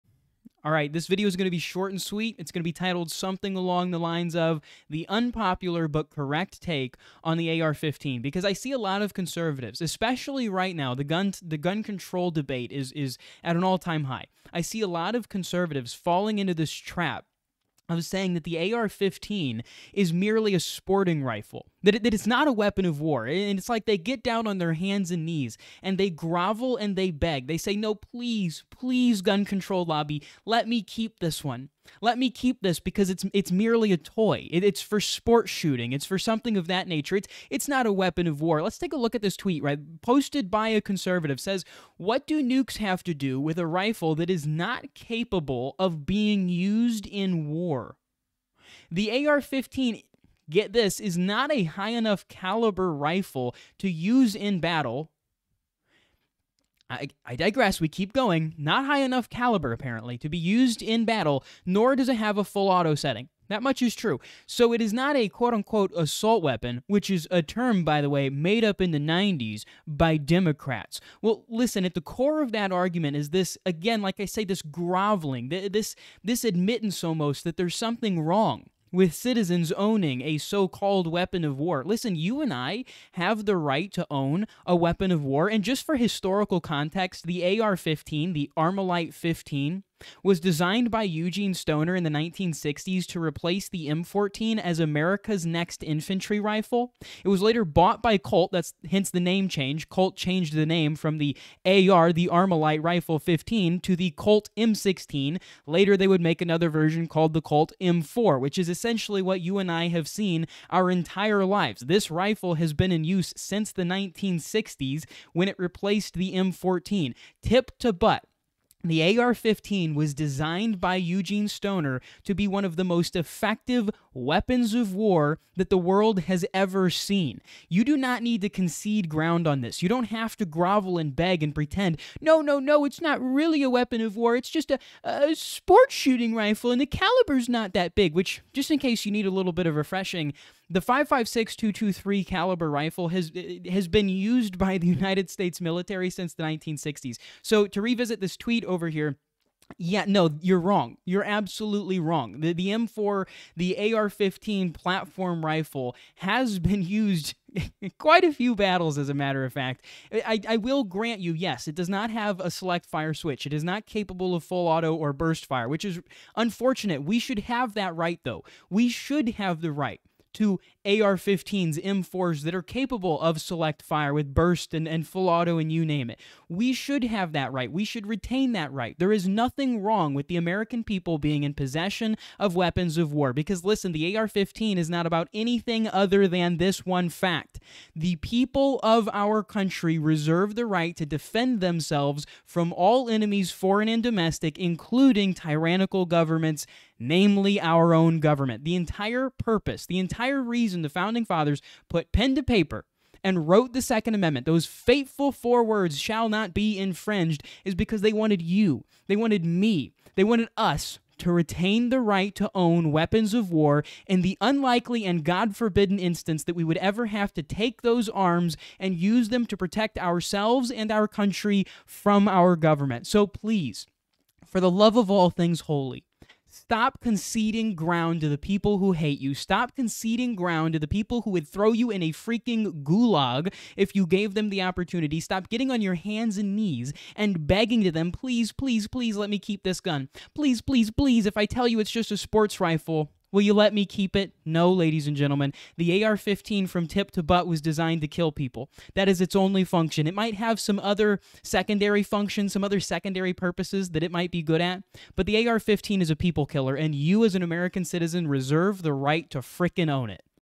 The Unpopular But Correct Take on the AR15 (Weapon of War?)
The AR15 was designed by Eugene Stoner to be one of the most effective killing machines in history..